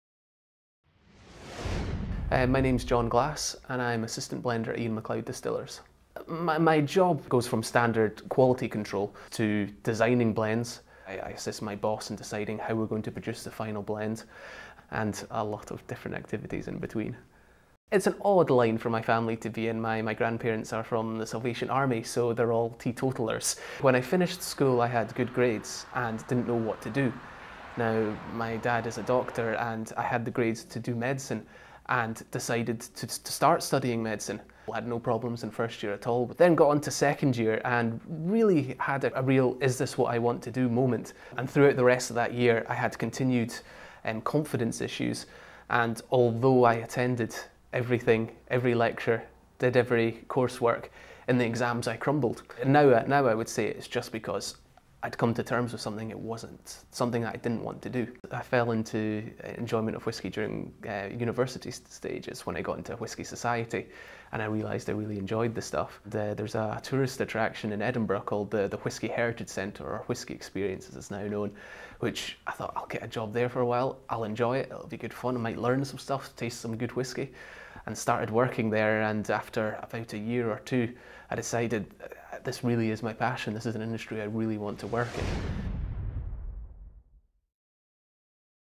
Career Advice on becoming an Assistant Blender by John G (Highlights)
for more careers info.

John G is an Assistant Blender at Glengoyne Distillery. In Medical School he realised he did not want to be a doctor. He changed to biochemistry and after graduating did not want to make a "hurried decision". He says "I fell into enjoyment of whisky during university". He has to smell and know the subtle difference between 200 different casks of whiskey every week.